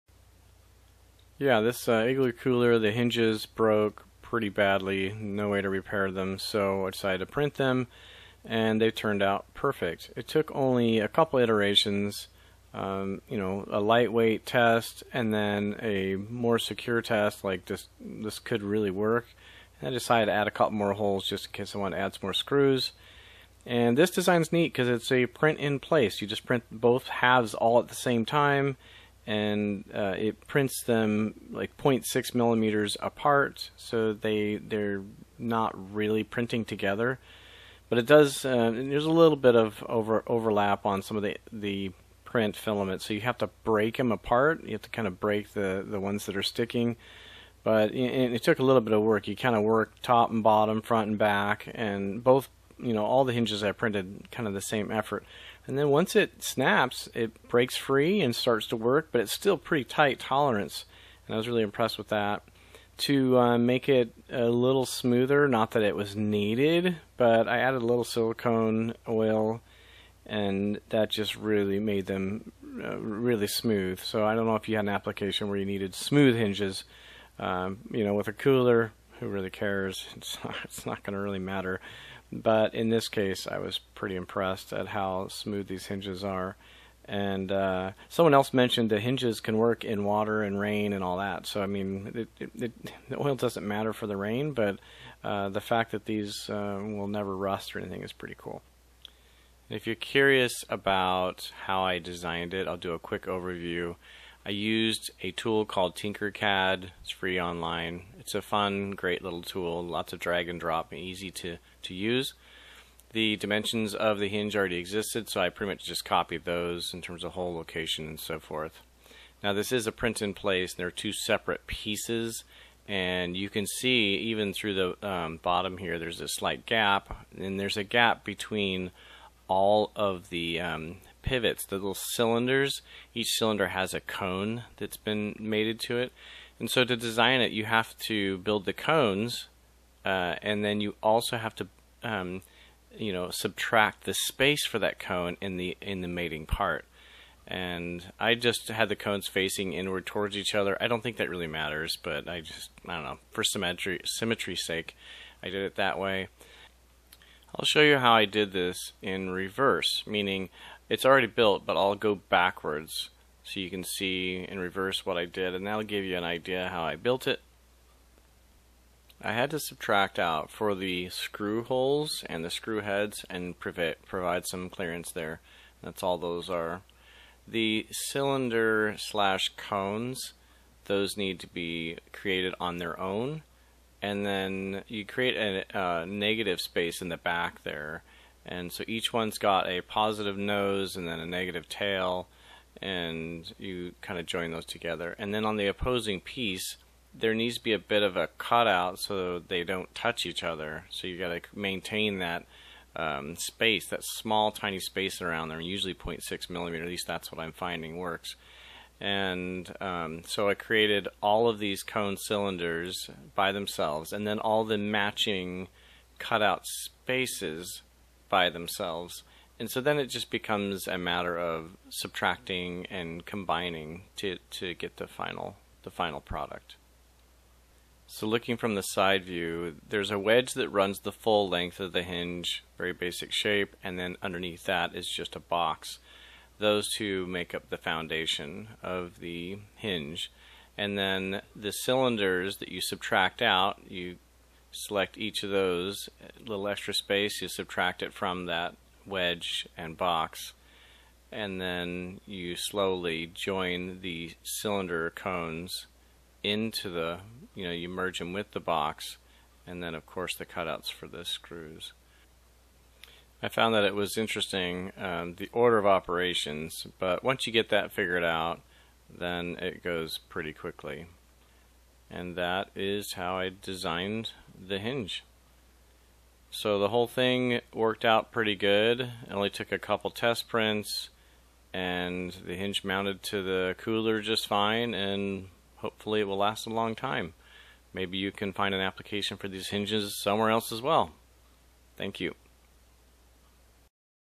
3D printed hinges (originally for Igloo cooler)
These are replacement hinges for the Igloo brand ice chest/cooler.
They are "print in place" hinges.
They could be useful for other applications as well.